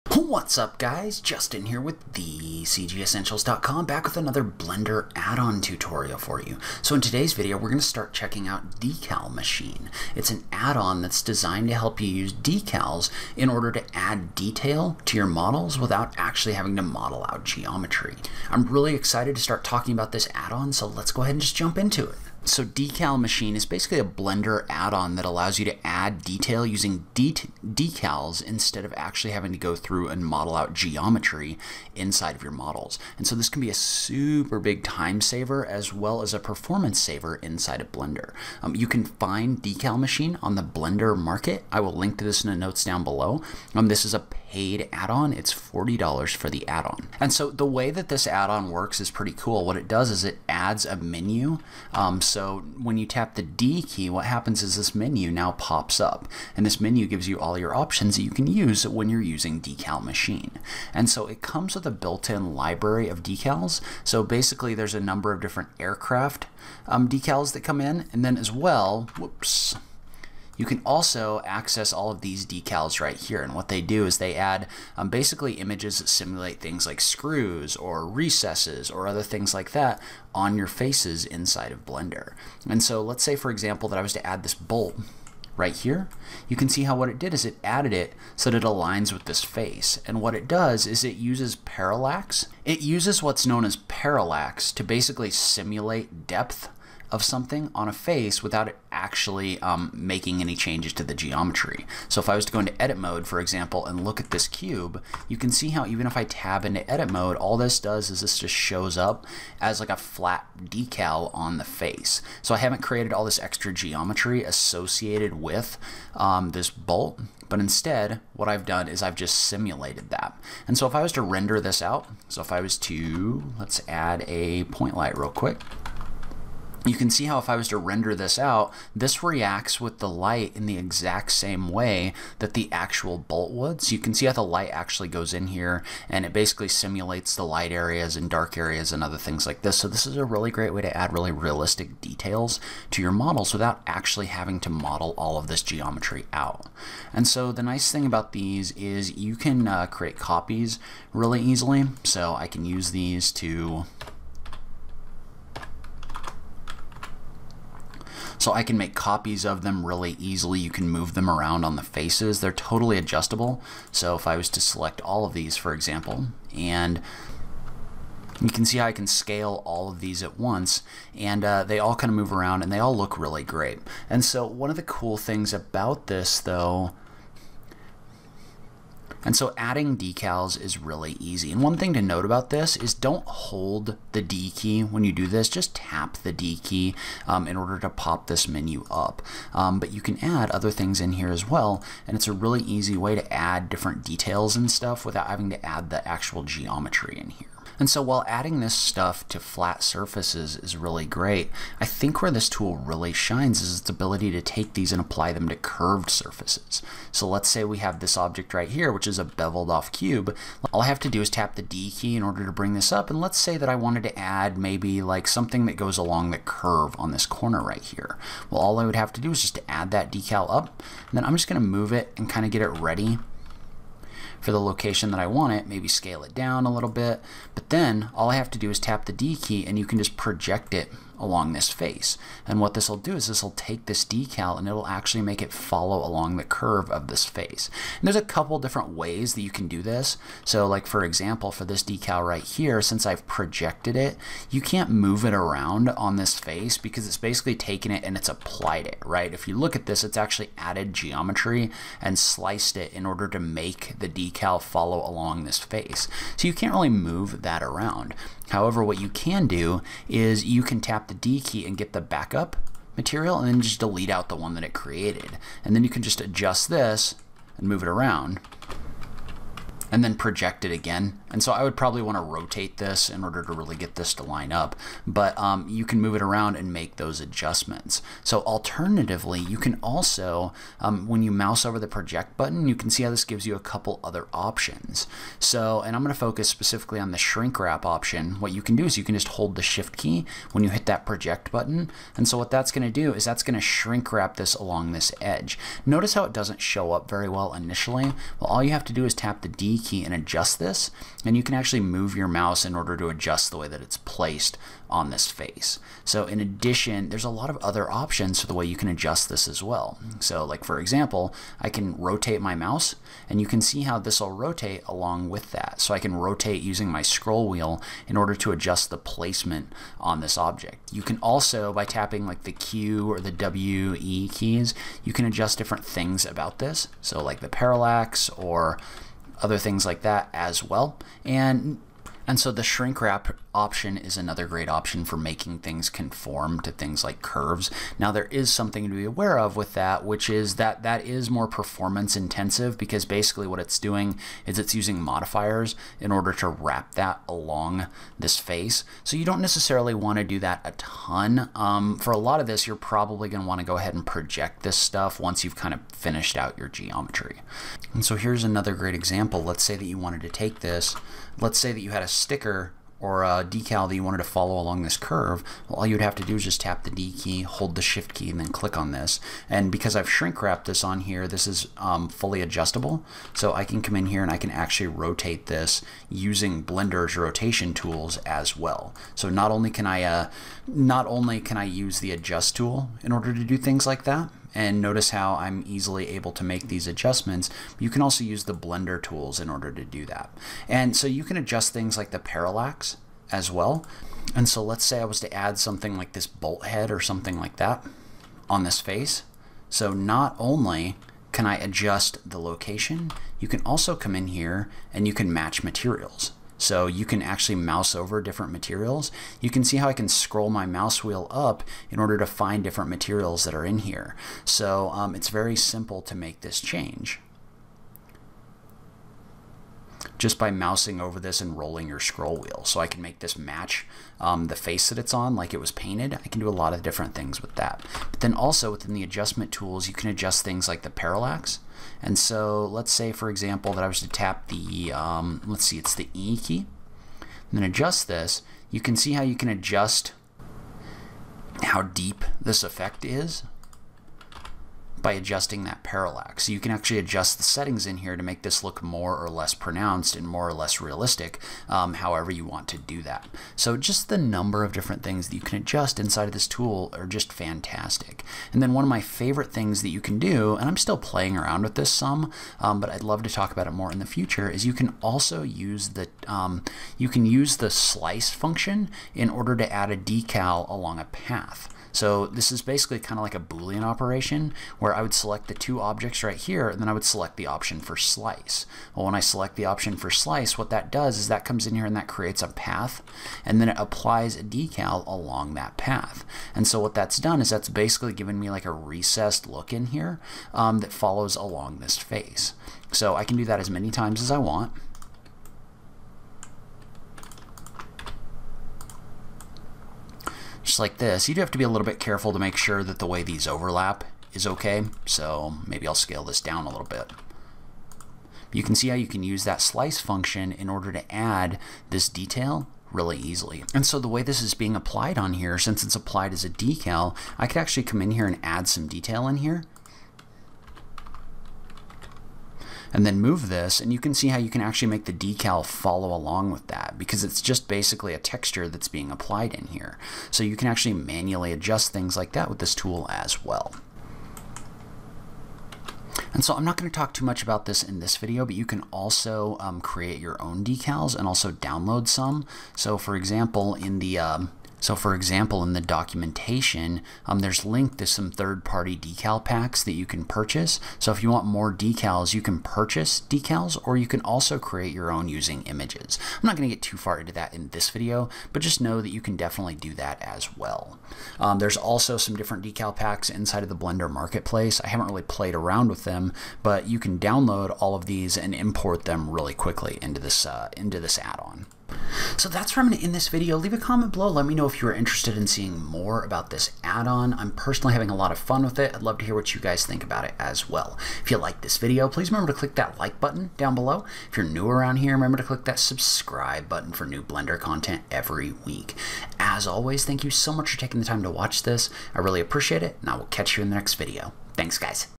Intro to DECAL MACHINE - the Amazing Blender Decal Add-On!
In today’s video, we’re going to start checking out an add-on for Blender that allows you to add amazing decals to your 3D models!

This add-on allows you to add detail to your models by applying to an image and using a UV offset to make the decals look more realistic.

I’ll give you kind of an overview of Decal Machine in this video, then if you’re interested, in the future we can get more in-depth about specific usages.

There’s a very comprehensive documentation website that contains a lot of information about the way the add-on works.

When you first load the add on, it adds an entirely new context menu that pops up when you press the “D” key. You can use the context menu in order to start adding decals.

Big note – don’t hold the “D” key – just tap it in order to get the menu to show up.

When you open up the menu, you have the ability to look through the library of built-in decals by clicking on the little boxes on the left. It comes with a library of aircraft decals and a number of example details that are great for adding bolts, hooks, and much more.

When you apply these to an object they look amazing. They use a shader effect called parallax to create the illusion of depth on your objects, so that when you render them, they look fully 3D without having to actually add the geometry.

This work great on flat faces, but where they really shine is when you start using them on faces that curve. This add-on will actually apply these decals to that they conform with your objects geometry, meaning they follow the curves of your object.

There are a few ways to do this – you can either project your object along a face, or your can also shrinkwrap by holding the shift key with this tool active. Note that the projected objects can’t really by moved, though you can pull the backup and move the decal around.

The shinkwrap method is adjustable, but can very quickly slow down your performance.